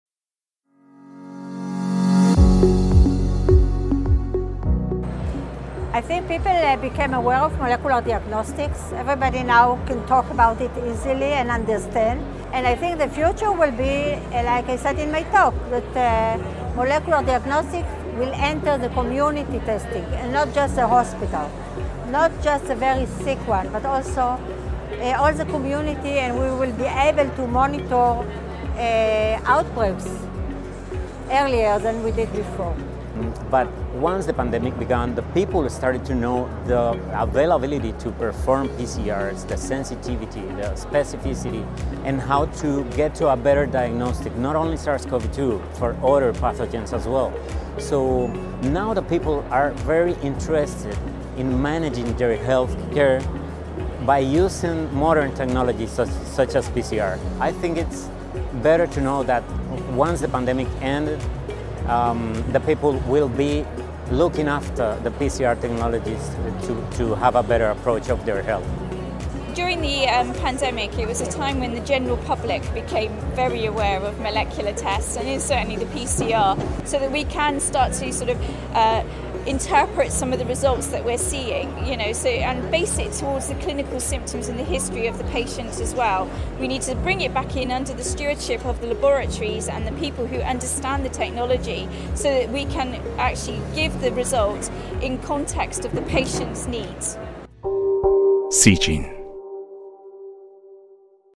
[2022 ECCMID] Healthcare Professionals Talking About Routine PCR Tests
Healthcare professionals from 2022 ECCMID talk about the importance of getting PCR tests routinely as we move towards to ‘living with COVID-19’ after the long-lasted pandemic.